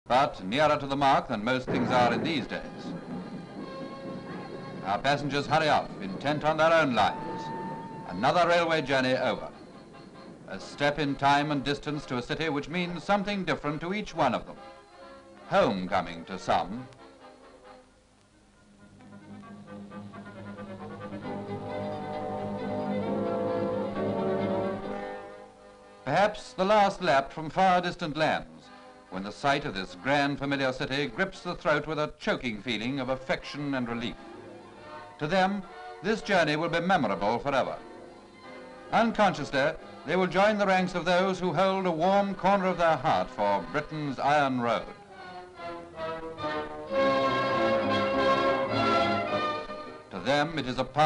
1946 Edinburgh Street Scenes and Station, 1940s UK Archive Footage
From the Kinolibrary archive film collections. To order the clip clean and high res Clip ref BPA3.1

1946 Edinburgh, street scenes, station